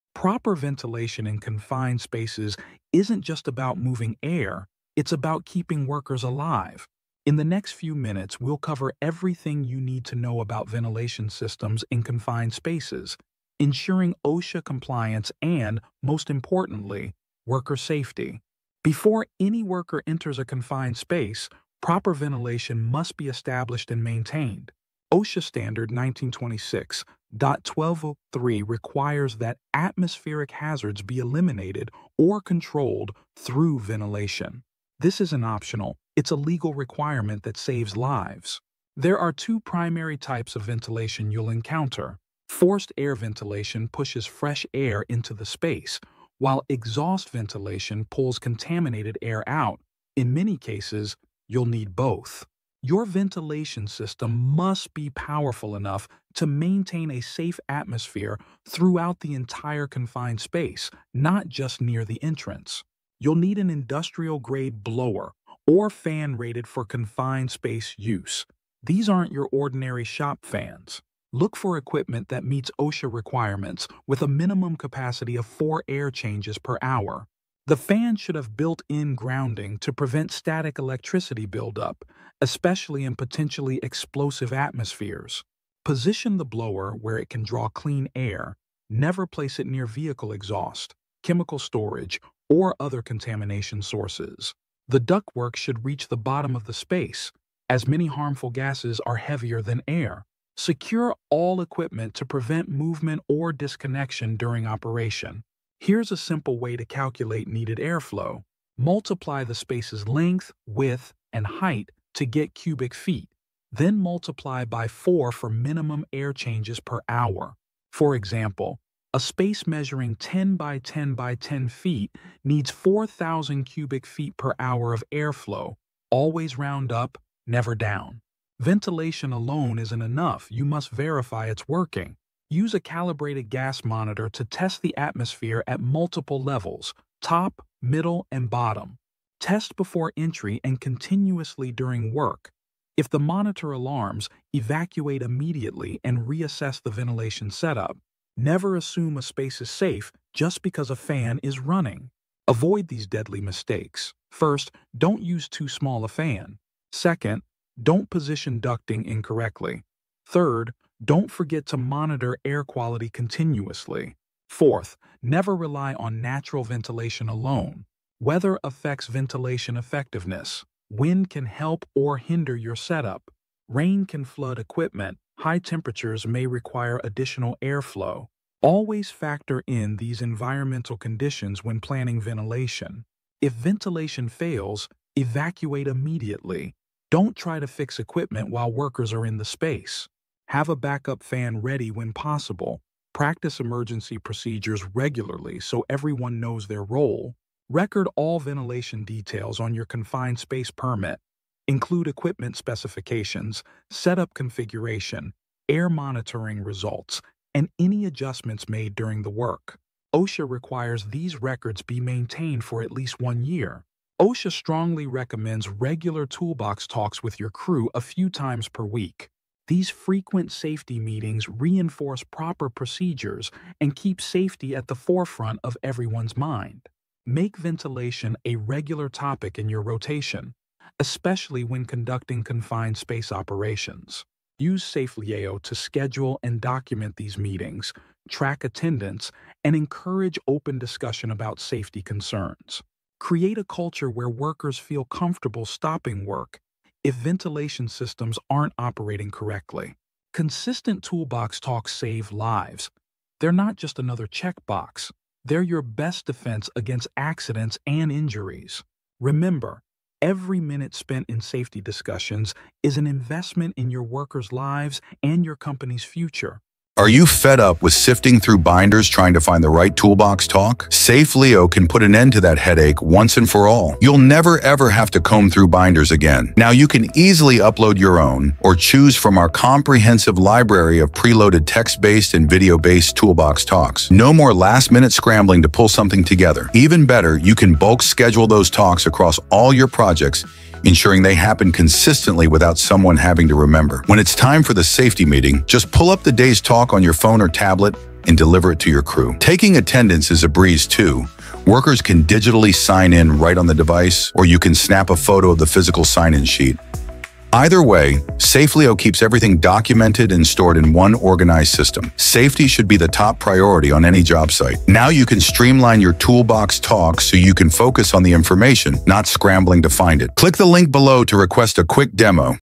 OSHA Confined Space Ventilation: A Quick Safety Guide
Learn confined space ventilation safety with our comprehensive guide designed for construction supervisors, safety managers, and workers. This essential training video breaks down OSHA-compliant ventilation requirements, equipment selection, and life-saving procedures for confined space operations. Whether you're new to confined space work or a seasoned professional, our step-by-step explanation will help you understand proper ventilation setup, monitoring requirements, and emergency procedures that could save lives on your job site.

Learn critical ventilation calculations, discover common mistakes that lead to accidents, and understand how to properly document your confined space operations. We cover everything from forced air and exhaust ventilation systems to real-world examples of what can go wrong when proper procedures aren't followed. This video includes essential information about air monitoring, equipment selection, and the importance of regular safety meetings, making it a must-watch for anyone involved in confined space work.

Don't risk workplace accidents or OSHA violations. Our expert-crafted content aligns with current OSHA standards and industry best practices, providing you with the knowledge needed to maintain a safe work environment. Make this video part of your regular safety training program and combine it with frequent toolbox talks to reinforce these critical safety procedures. Remember, proper confined space ventilation isn't just about compliance, it's about ensuring every worker goes home safely at the end of their shift.